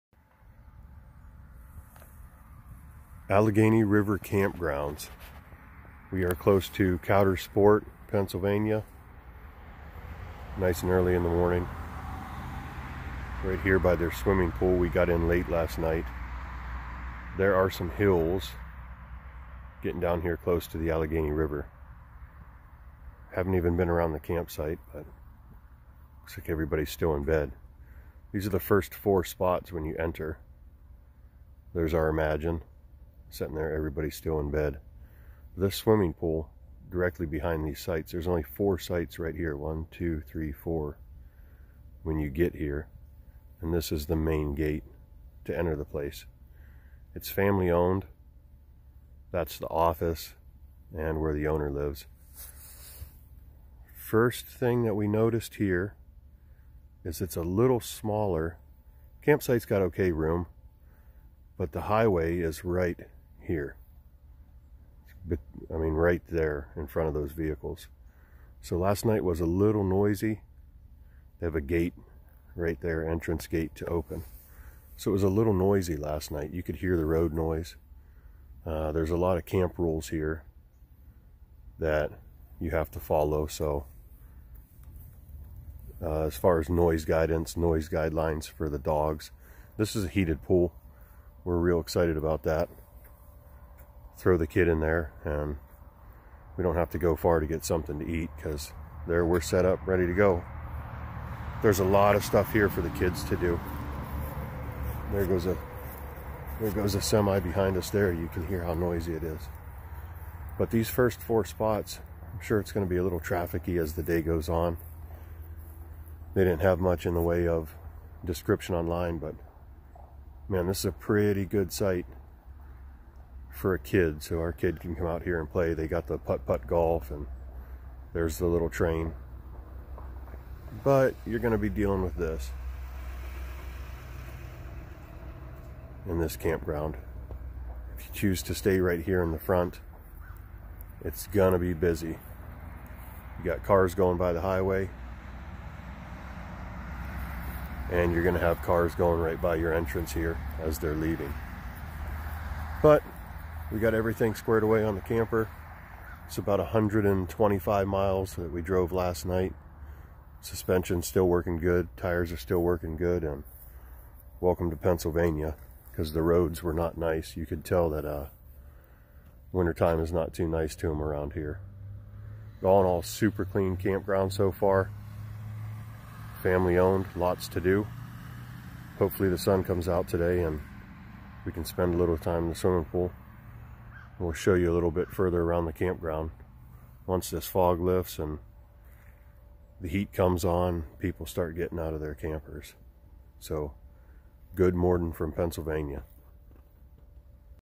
Allegheny River Campground
Grand Design Imagine 2800bh, F250. Campground first look. Allegheny River Camping near Kinzua Dam. Family owned campground with full hookups. $55 a day for a site. Room for the kids to play. Heart of the Allegheny National Forest.